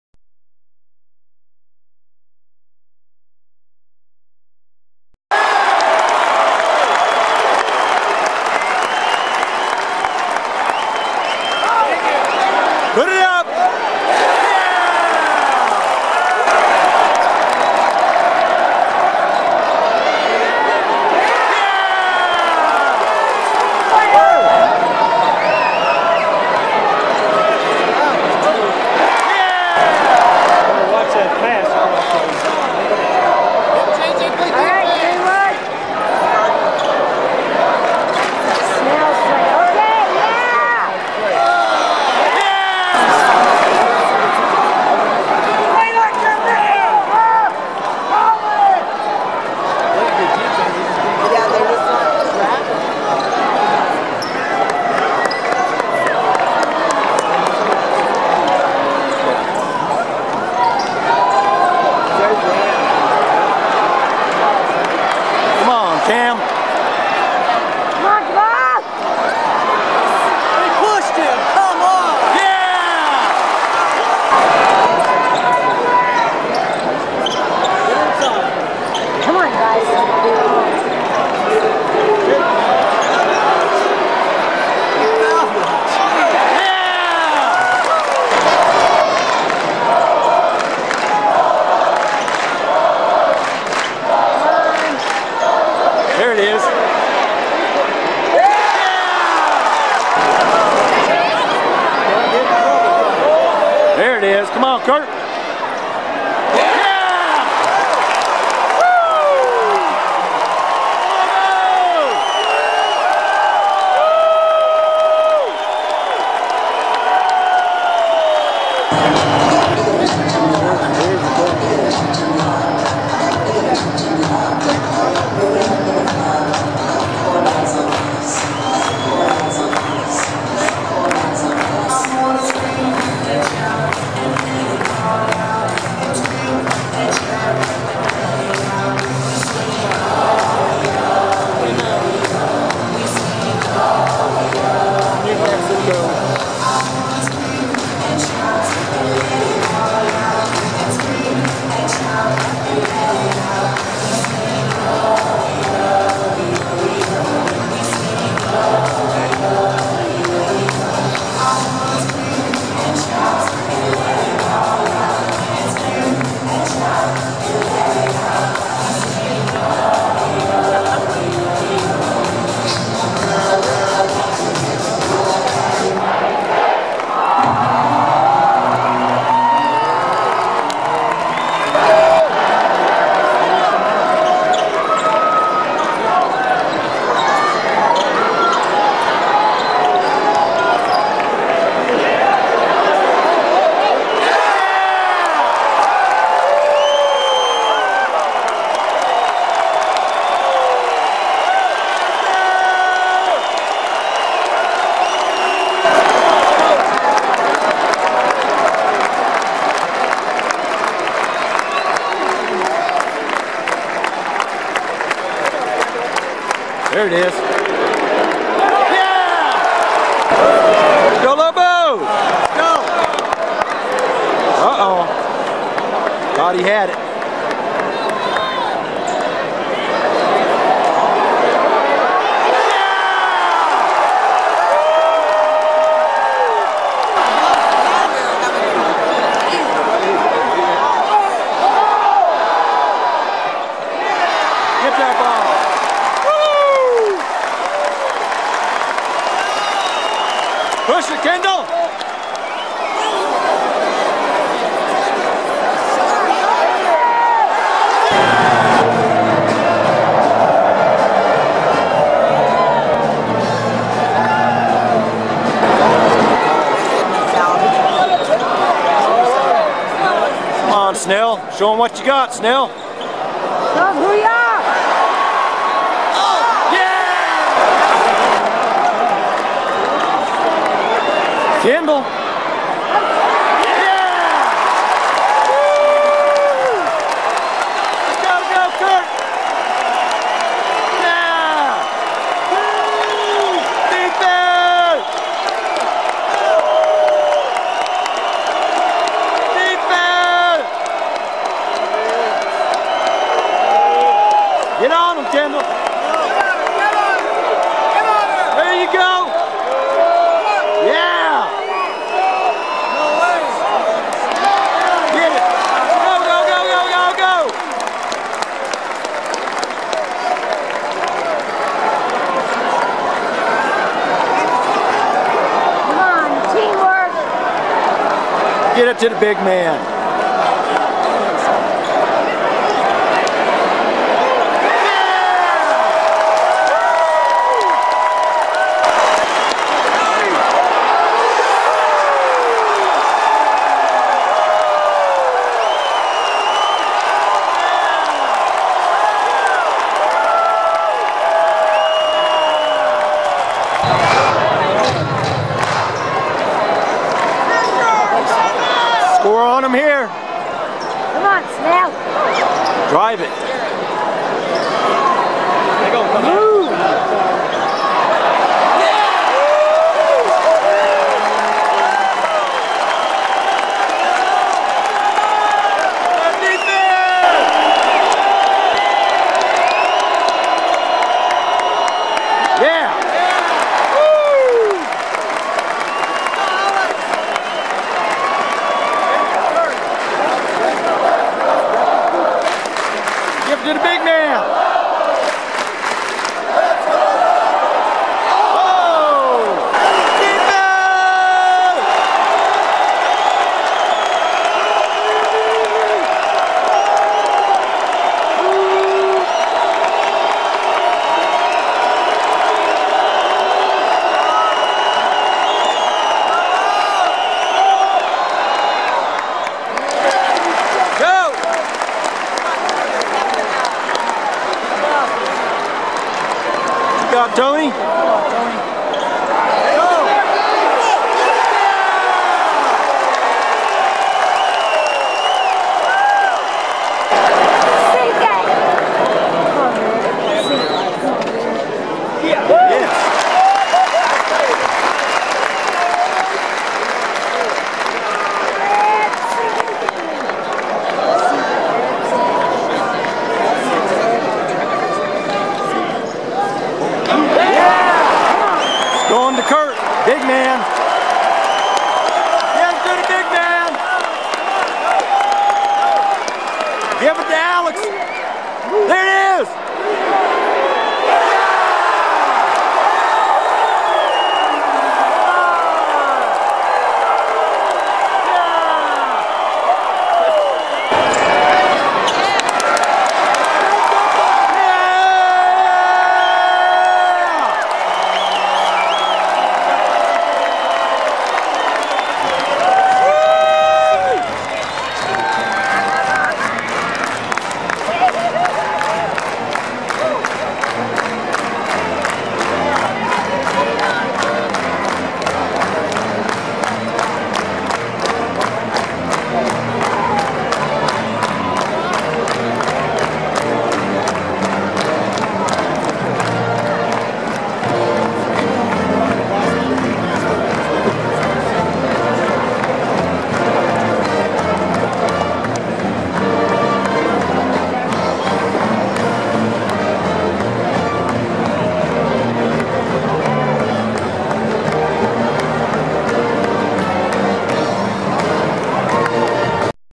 SDSU vs. UNM Highlights at the "PIT"
SDSU vs. UNM Highlights at the PIT I shot this video on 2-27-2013 The Lobos won the game 70-60 with a record of 24-4, and they're ranked #14 in the nation!!